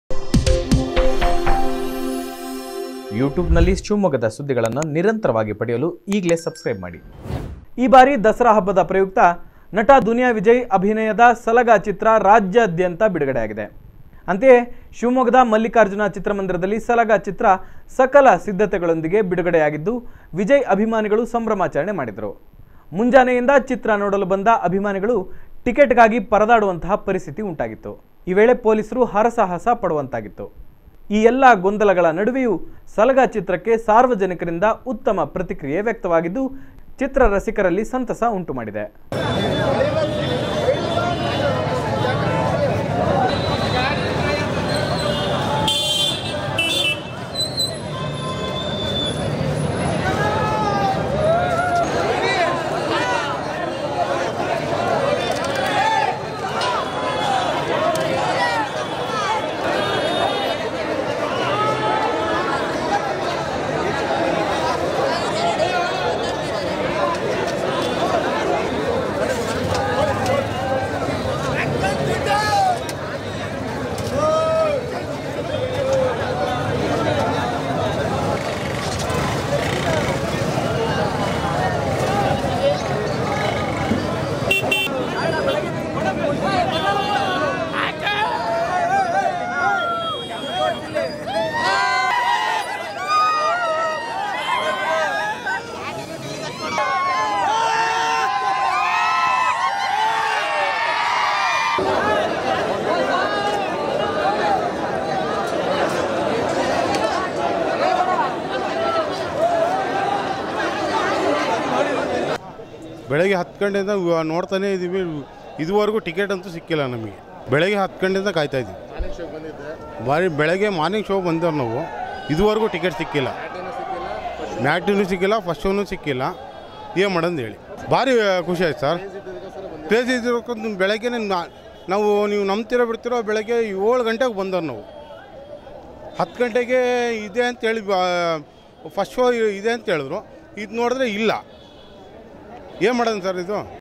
ಸಲಗ ಚಿತ್ರಕ್ಕೆ ಗುಡ್ ರೆಸ್ಪಾನ್ಸ್ Good response to the movie Salga!
ಈ ಬಾರಿ ದಸರಾ ಹಬ್ಬದ ಪ್ರಯುಕ್ತ ನಟ ದುನಿಯಾ ವಿಜಯ್ ಅಭಿನಯದ ಸಲಗ ಚಿತ್ರ  ರಾಜ್ಯಾದ್ಯಂತ ಬಿಡುಗಡೆಯಾಗಿದೆ.  ಅಂತೆಯೇ ಶಿವಮೊಗ್ಗದ ಮಲ್ಲಿಕಾರ್ಜುನ ಚಿತ್ರಮಂದಿರದಲ್ಲಿ  ಸಲಗ ಚಿತ್ರ ಸಕಲ ಸಿದ್ಧತೆಗಳೊಂದಿಗೆ  ಬಿಡುಗಡೆಯಾಗಿದ್ದು ವಿಜಯ್ ಅಭಿಮಾನಿಗಳು ಸಂಭ್ರಮಾಚರಣೆ ಮಾಡಿದರು.  ಮುಂಜಾನೆಯಿಂದ ಚಿತ್ರ ನೋಡಲು ಬಂದ ಅಭಿಮಾನಿಗಳು ಟಿಕೆಟ್ಗಾಗಿ ಪರದಾಡುವ ಪರಿಸ್ಥಿತಿ ಉಂಟಾಗಿತ್ತು, ಈ ವೇಳೆ ಪೊಲೀಸರು ಹರಸಾಹಸ ಪಡುವಂತಾಗಿತ್ತು. ಈ  ಎಲ್ಲ ಗೊಂದಲಗಳ ನಡುವೆಯೂ ಸಲಗ ಚಿತ್ರಕ್ಕೆ ಸಾರ್ವಜನಿಕರಿಂದ ಉತ್ತಮ ಪ್ರತಿಕ್ರಿಯೆ ವ್ಯಕ್ತವಾಗಿದ್ದು  ಚಿತ್ರರಸಿಕರಲ್ಲಿ ಸಂತಸ ಉಂಟು ಮಾಡಿದೆ.